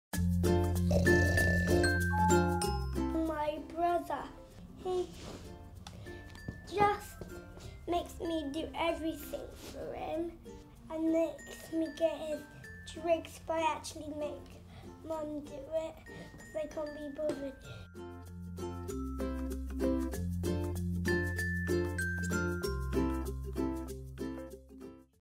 Meet Our... Mr. Lazy
My 1st Years asked some big characters which Mr. Men Little Miss character they think best represent them and their family... say hello to our Mr. Lazy.

This video is to celebrate the launch of My 1st Years' very exciting collaboration with the Mr. Men Little Miss Characters. Discover your personalised clothing, accessories and furniture from the collection